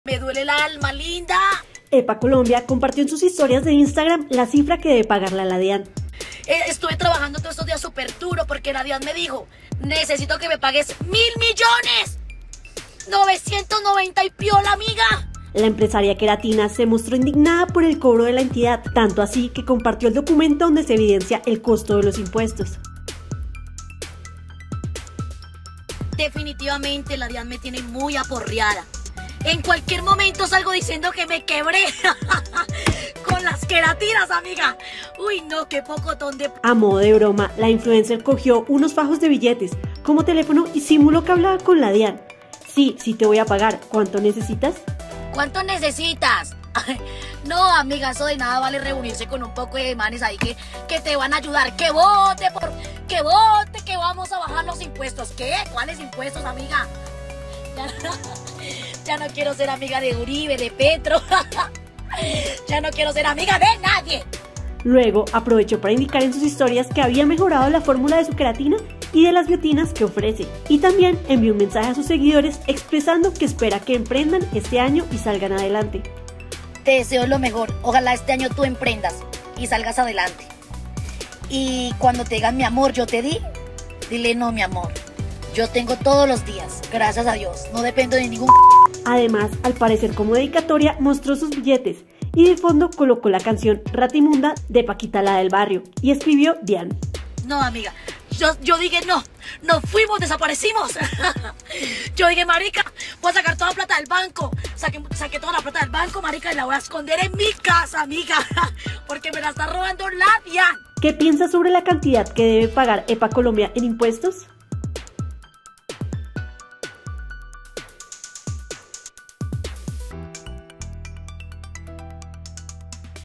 Epa Colombia reveló todo lo que le cobra la DIAN
Epa Colombia se mostró indignada al compartir con sus seguidores la suma que le debe pagar a la DIAN. Esta es la cifra.